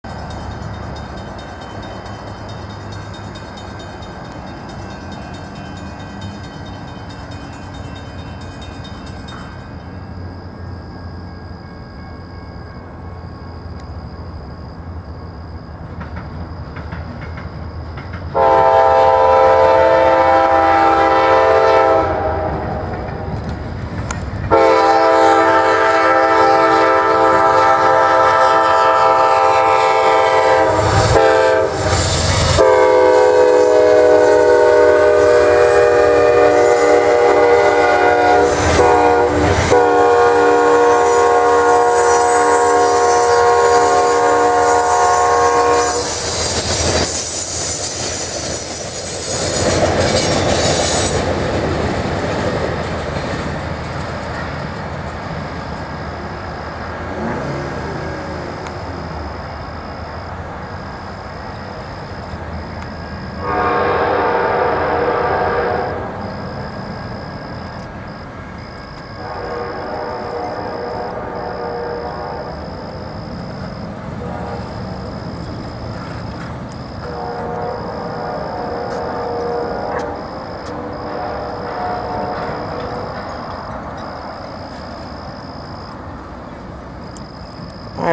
The Amtrak Crescent 19 Westbound In(HD) Austell,Ga. 7-29-2013
The Amtrak Crescent #19 Westbound The Grand Prize Of The Day This Is What Every BODY COME TO SEE.
AMTK 164-P42DC
AMTK 118-P42DC
09:07AM Monday